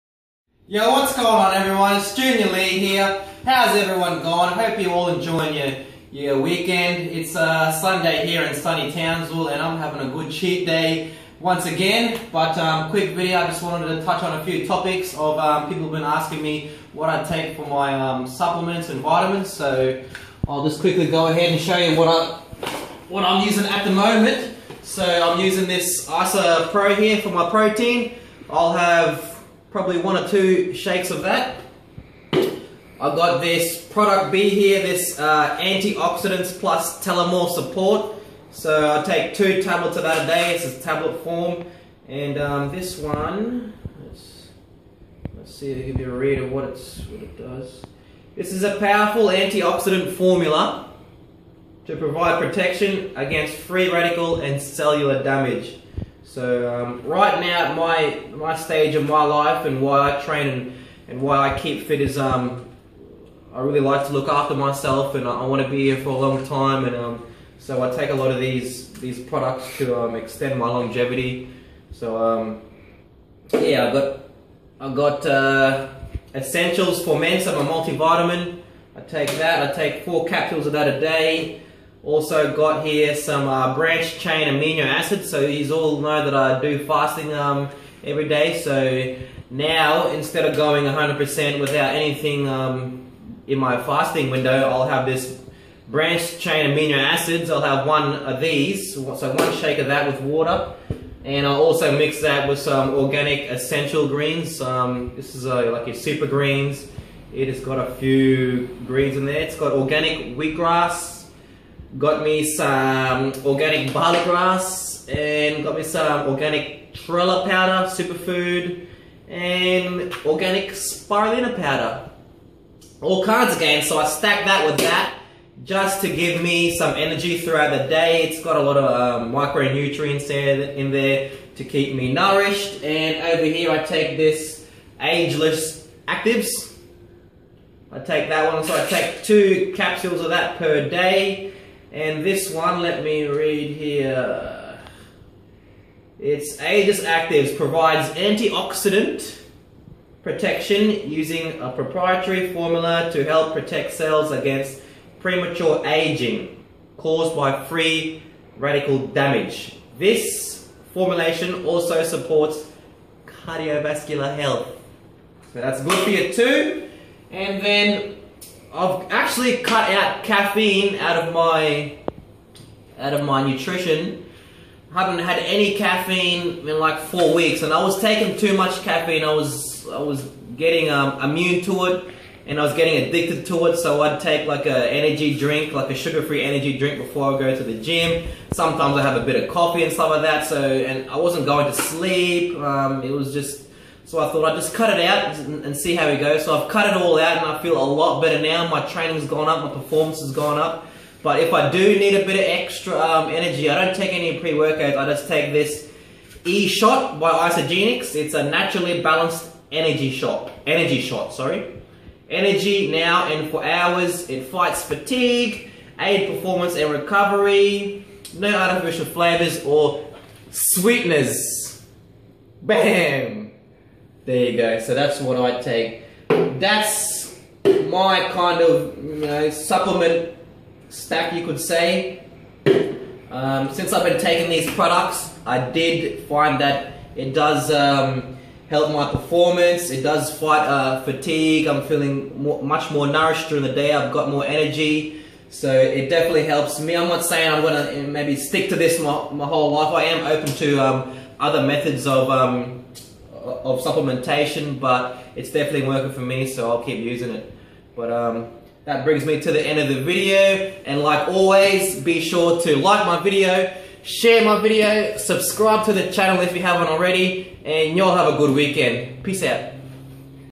My current supplementation
This is a look into my current supplementation and some of the products i like to use. I have experienced differences in my overall health & fitness as result in using these products. If you have not looked over your own current supplementation lately or have been lacking in performance I would invite you to do so.